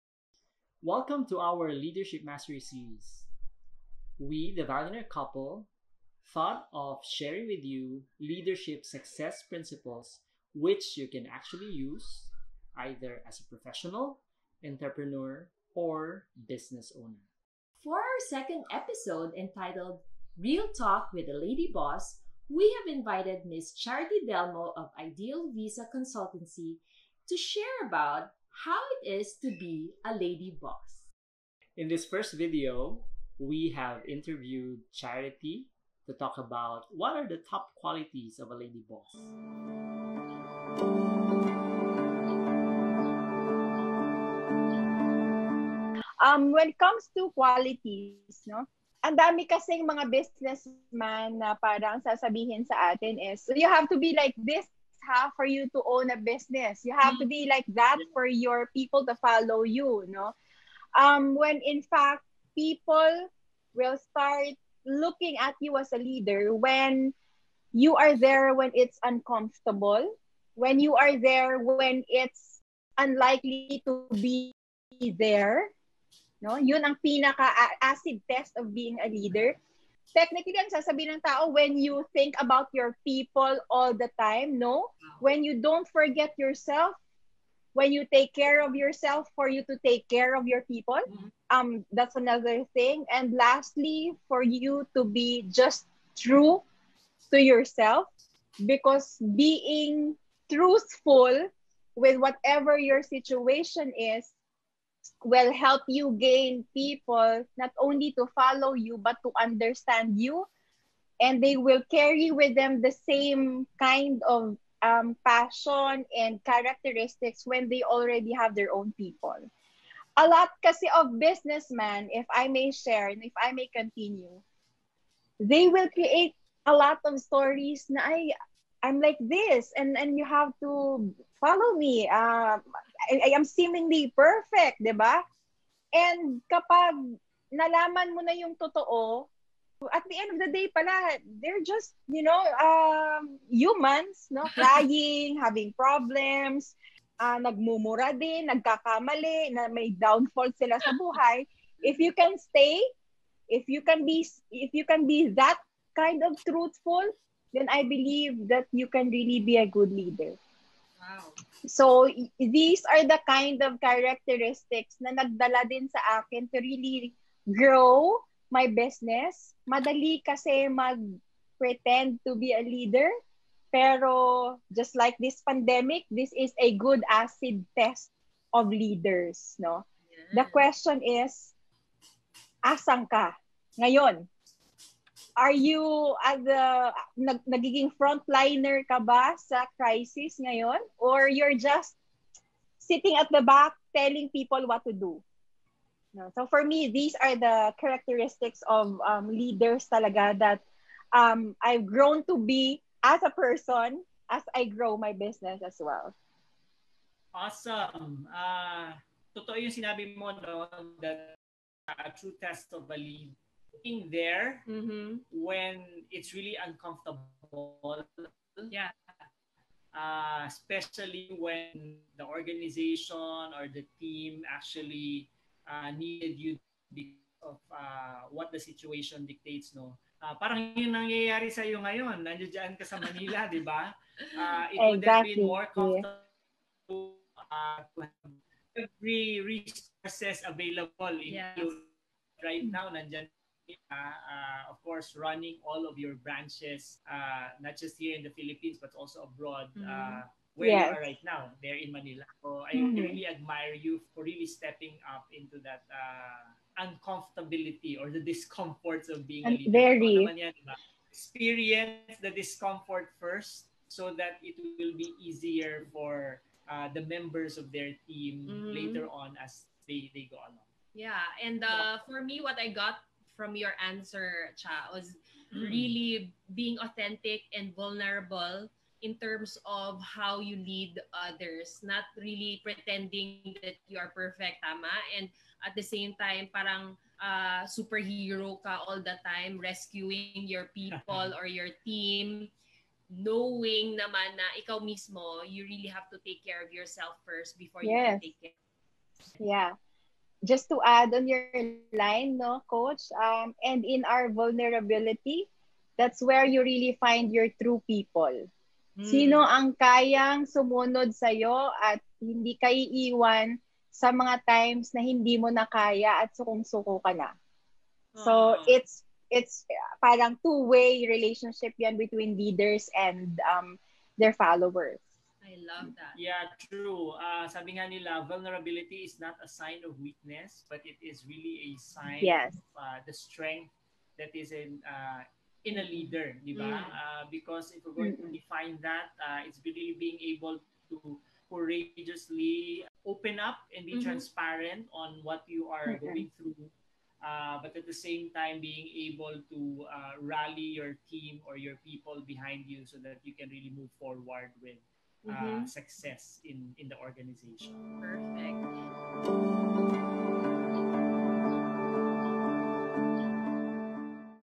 TOP QUALITIES YOU NEED TO BE A LADY BOSS (Real Talk with a Lady Boss)
This video is part of The Valuenaire Couple's Leadership Mastery Series featuring REAL TALK WITH A LADY BOSS.

For this episode we have invited MS. CHARITY DELMO, Founder of IDEAL VISA CONSULTANCY. Her team and her company helped a lot of Filipinos fulfill their dreams of studying and working abroad through the services they provide.

Listen and learn from this interview as we discover how a lady boss leads her nationwide and international operations despite the many challenges in running a business especially during these times.